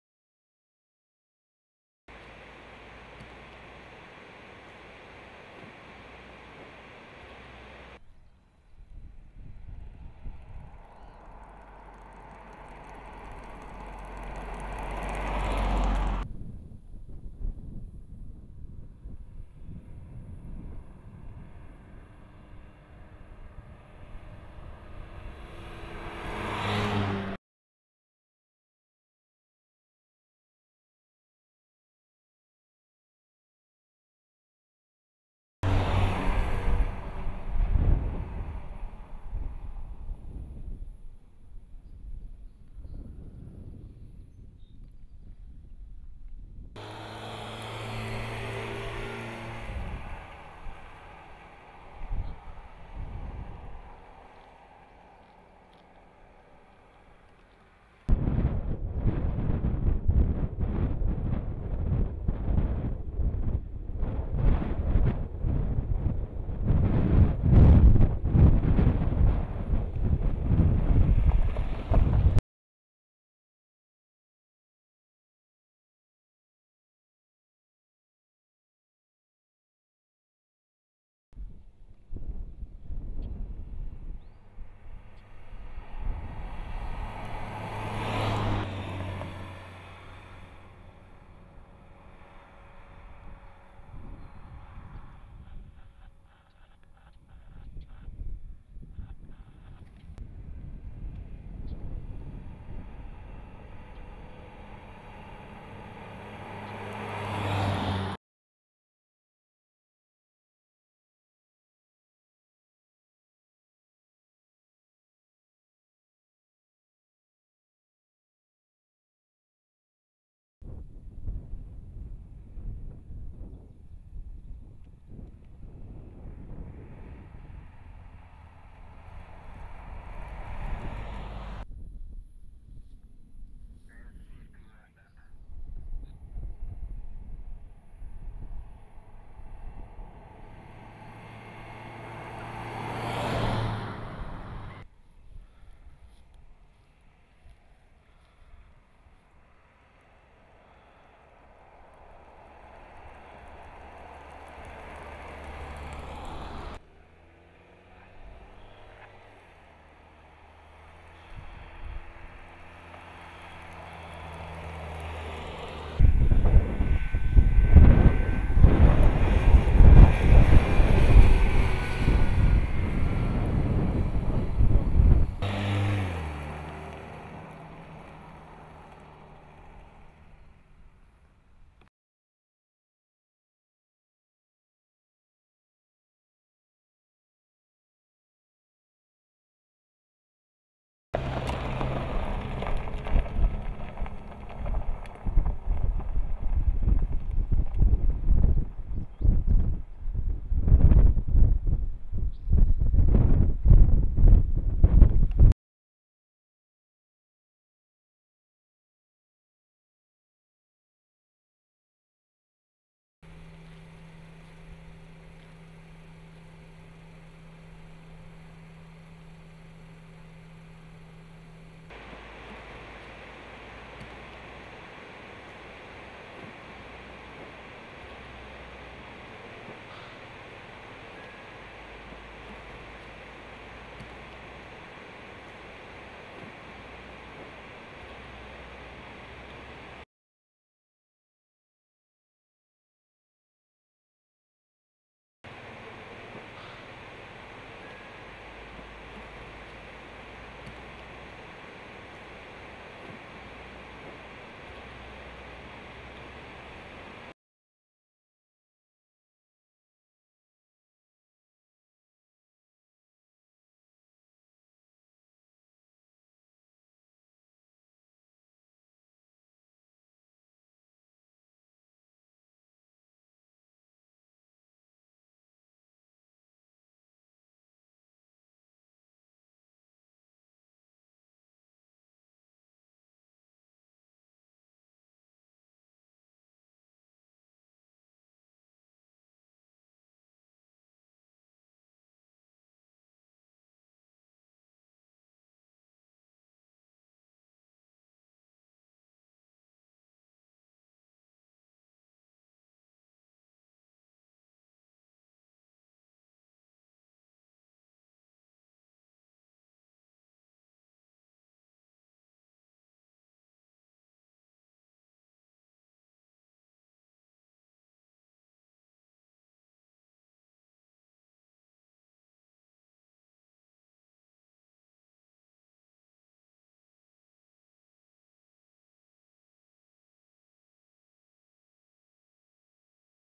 2017 Jeep Compass Limited Running Footage
The 2017 Jeep® Compass is available in four different trim configurations: Sport, Latitude, Limited and Trailhawk.